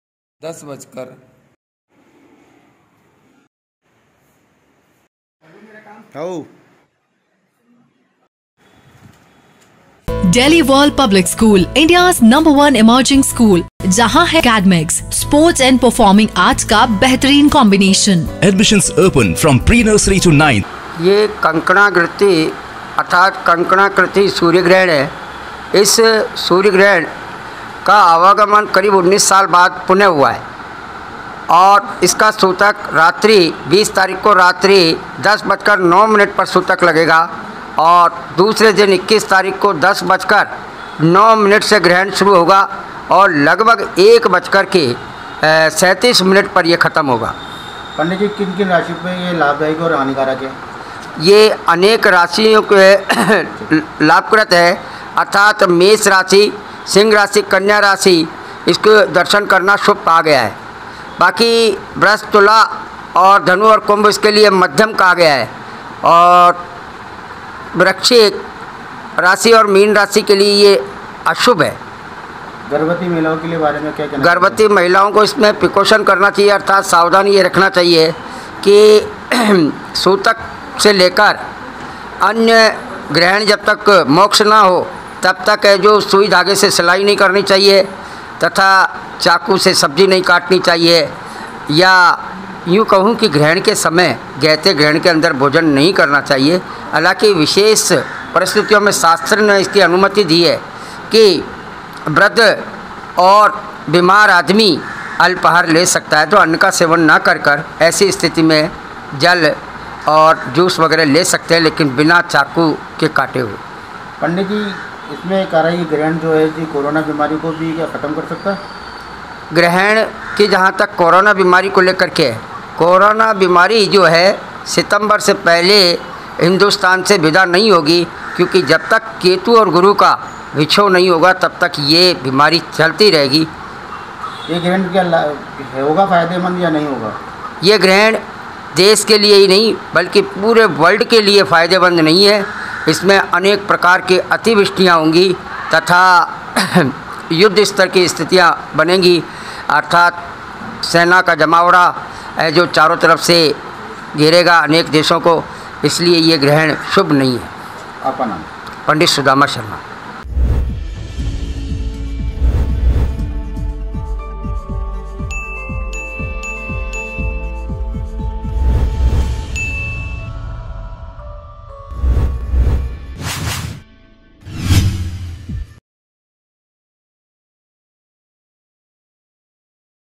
Ajmer | रविवार को साल का पहला सूर्यग्रहण | MTTV INDIA
रविवार को साल का पहला सूर्यग्रहण
आषाढ़ मास के कृष्ण-पक्ष की अमावस्या को लगेगा ग्रहण
पंडित सुदामा ने सूर्यग्रहण के बारे में बताया
3 घंटे 34 मिनट का रहेगा ग्रहण